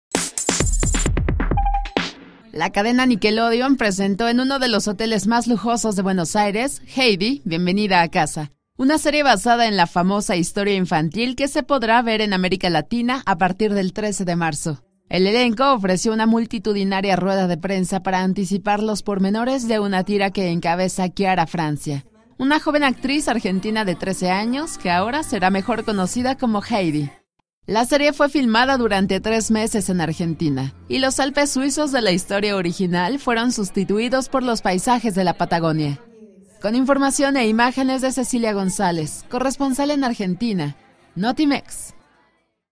Lanza Nickelodeon "Heidi". bienvenida a casa" en América Latina
La serie, filmada durante tres meses en Argentina, sustituyó los Alpes suizos de la historia original por los paisajes de la Patagonia